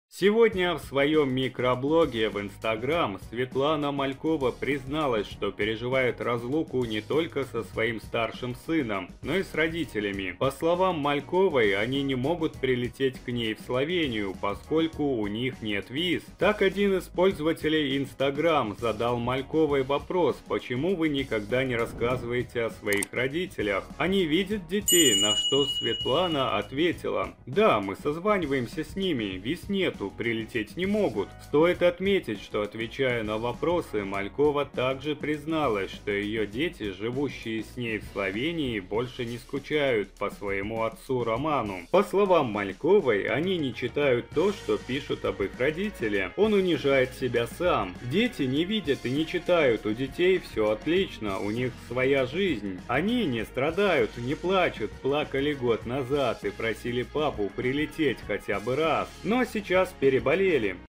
Светлана Малькова призналась, что не видится со своими родителями.
Светлана Малькова рассказала, что пишет книгу об истории своего развода